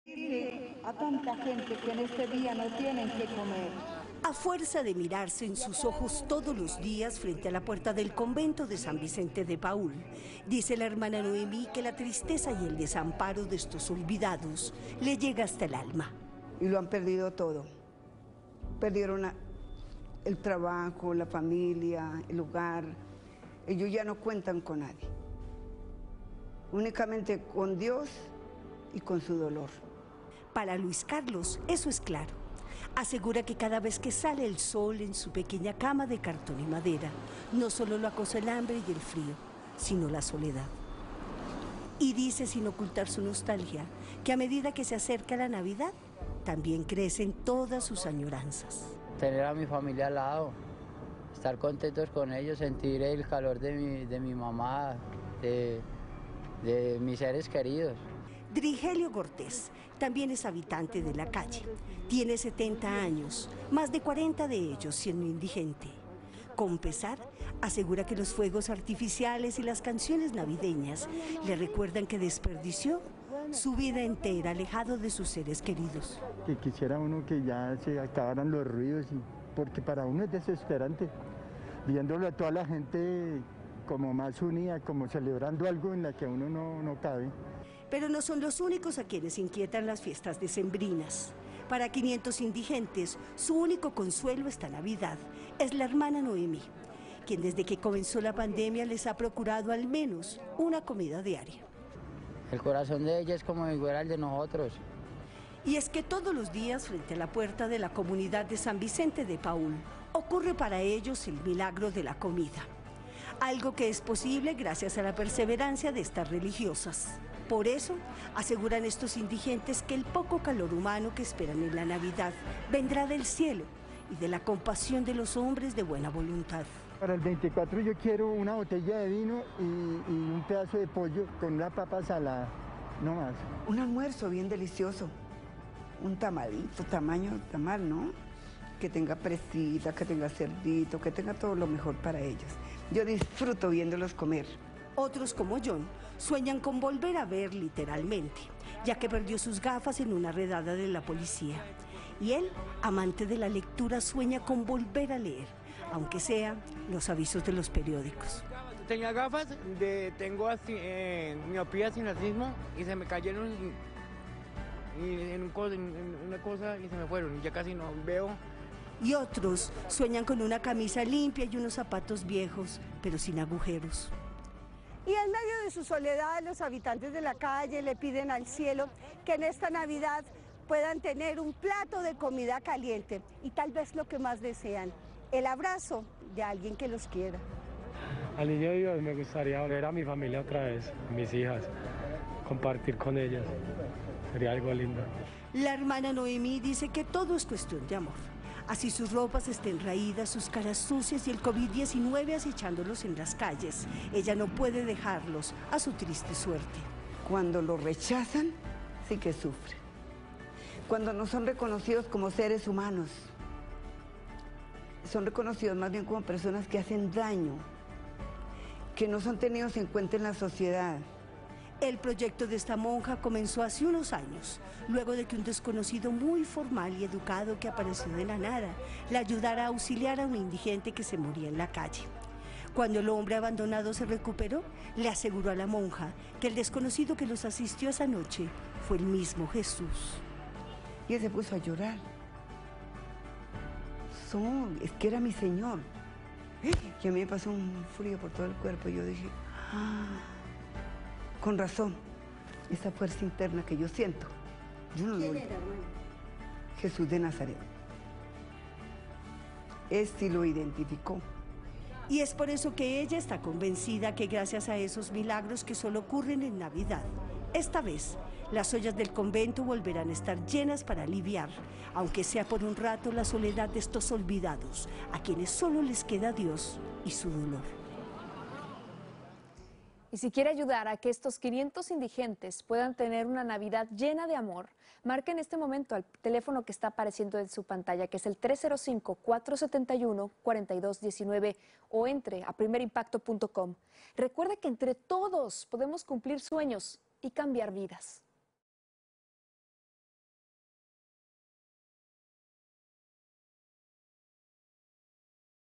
Sueño Navideños: Esta monja pide ayuda para alimentar a más de 500 indigentes
La hermana Nohemí ha ayudado a decenas de desamparados en las calles de Bogotá, Colombia. Pero por la crisis en la pandemia muchas otras personas han llegado para pedir un plato de comida. Ya son más de 500 las que necesitan alimentos.